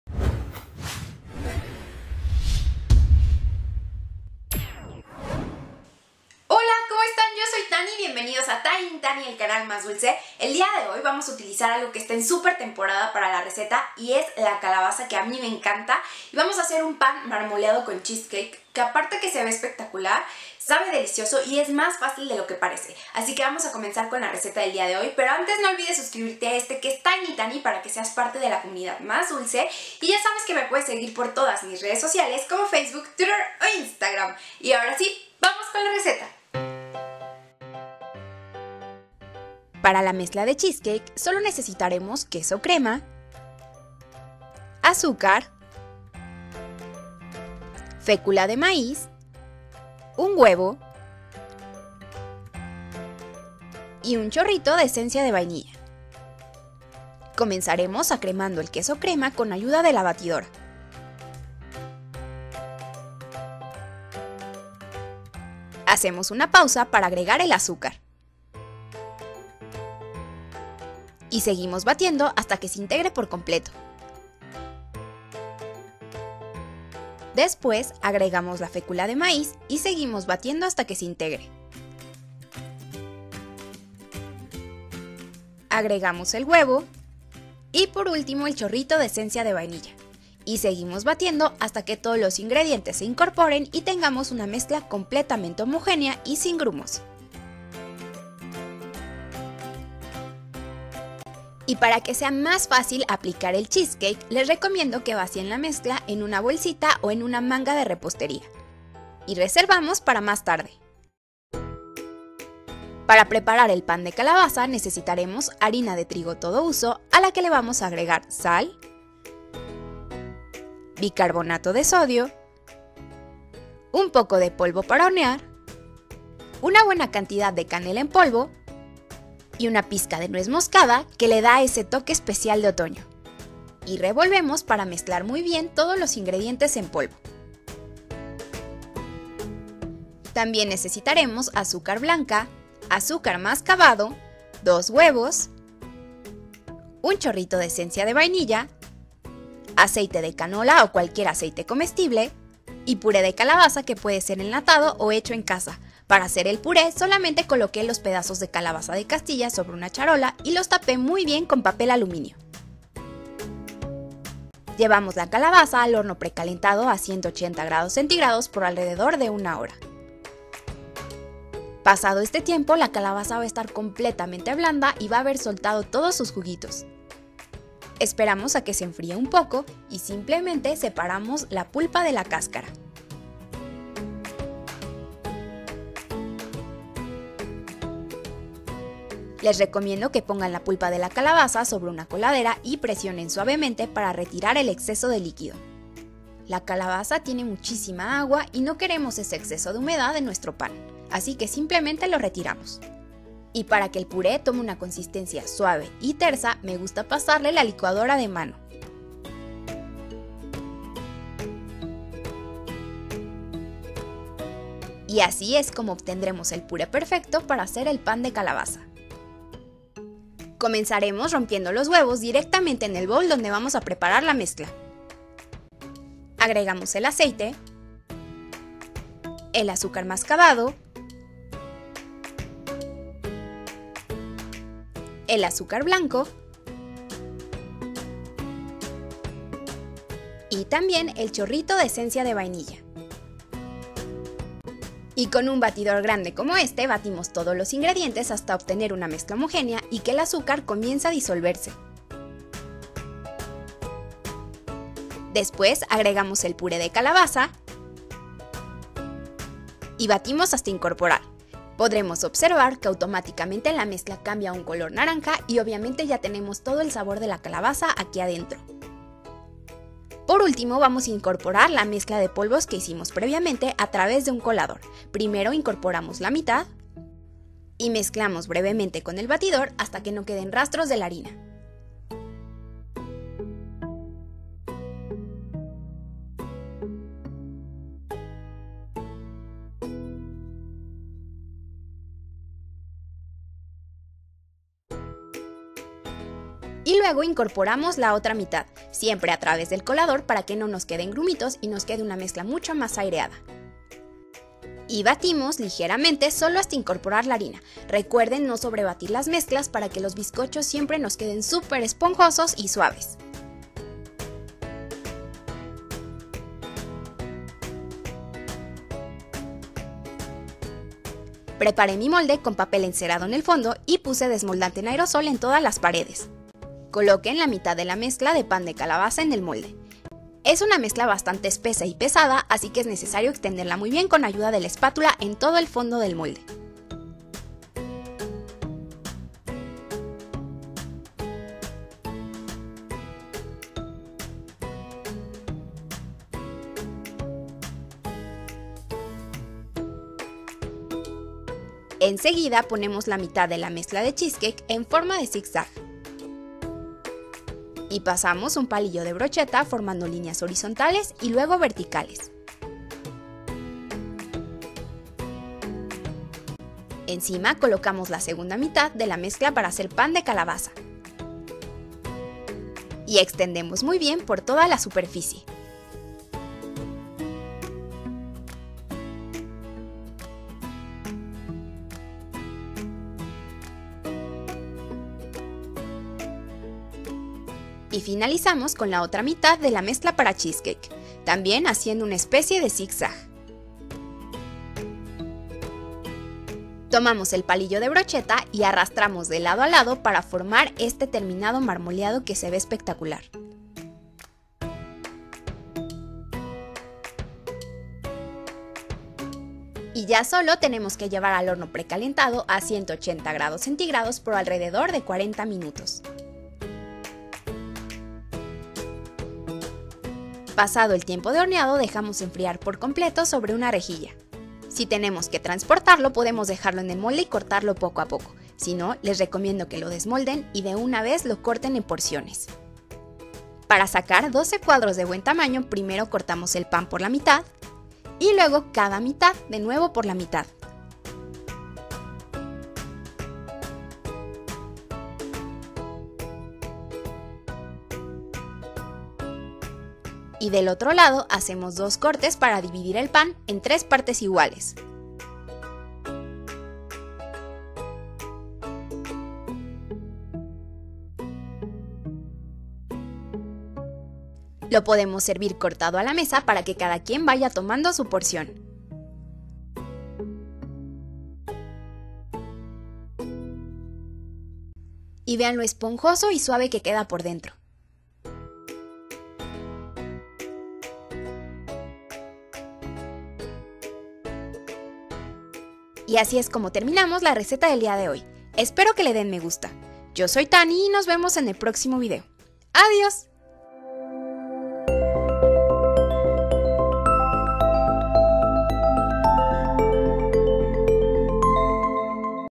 Pan de Calabaza Marmoleado con Cheesecake 🍁
Las calabazas son un ícono del otoño, por eso les enseño a preparar un delicioso y fácil pan de calabaza con betas de cheesecake para dar ese efecto marmoleado que se ve espectacular y se logra con el mínimo esfuerzo.

INGREDIENTES
(molde 15x25 cm)

-Cheesecake
190g de queso crema
50g de azúcar
5g de fécula de maíz
1 huevo
5ml de esencia de vainilla

-Pan de Calabaza
215g de harina
1/2 cdita. de sal
1 cdita. de bicarbonato de sodio
1/2 cdita. de polvo para hornear
1 1/2 cditas. de canela en polvo
1/2 cdita. de nuez moscada
80g de azúcar blanco
80g de azúcar mascabado
2 huevos
220g de puré de calabaza (puede ser enlatado o hecho en casa)
100ml de aceite de canola (o cualquier aceite vegetal de sabor suave)
7ml de esencia de vainilla